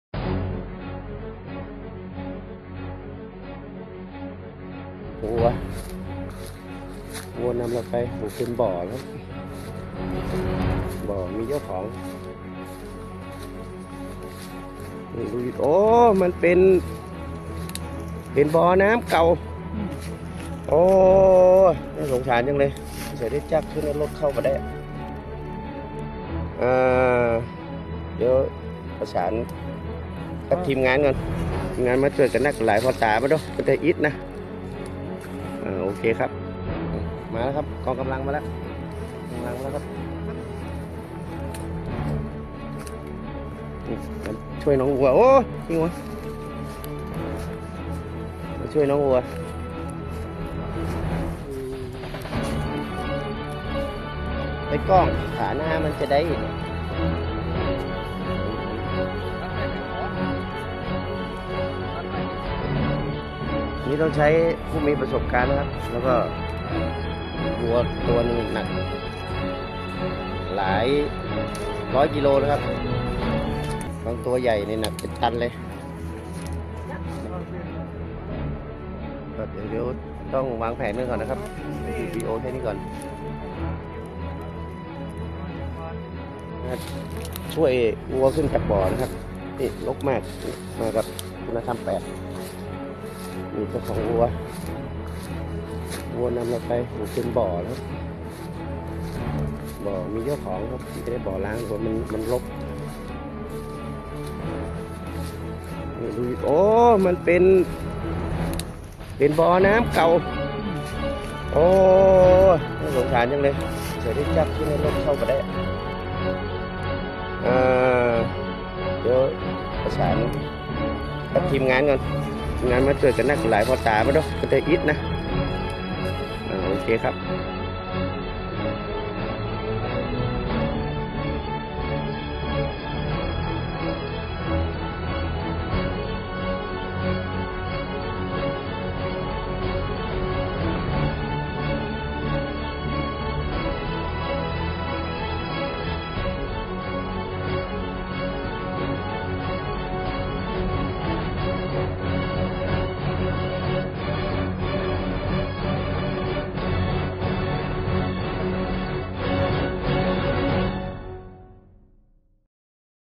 พะเยา ระดมคนรถแบคโฮ ช่วยวัวตกบ่อน้ำร้างกลางทุ่งนา
นายทอน แก้วชาญ อายุ60 ปีได้นำวัวออกเลี้ยงกลางทุ่งนา บ้านสันป่าตอง ม.2 ต. สันโค้งอำเภอดอกคำใต้จังหวัดพะเยา แล้ววัวหายไปจึงออกตามหา พบว่าวัวพลัดหลง เข้าไปในสวนของชาวบ้านแล้วตกในบ่อน้ำร้างลึก3เมตร ขึ้นไม่ได้
หลังจากนั้นจึงรีบวิ่งไป บอกเจ้าหน้าที่ งานป้องกัน และบรรเทาสาธารณภัย อบต.สันโค้ง มาช่วยเหลือนำวัวขึ้นจากบ่อน้ำด้วย หลังรับแจ้งจึงนำกำลังคนไปช่วยนำวัวขึ้นจากบ่อน้ำแต่เอาวัวขึ้นไม่ได้ เพราะบ่อน้ำลึก 3เมตรปากบ่อกว้าง2เมตรและตัววัวหนักหลายร้อยกิโลกรัม จึงนำ รถแบคโฮ มาทำการขึดปากบ่อ และดึงชักลากวัวขึ้นมาได้จนสำเร็จโดยวัวปลอดภัยไร้อาการบาดเจ็บแต่อย่างใด  ท่ามกลางความดีใจของเจ้าของวัวและ เจ้าหน้าที่ชุดกู้ภัยปภ .อบต.สันโค้ง ไปตามกัน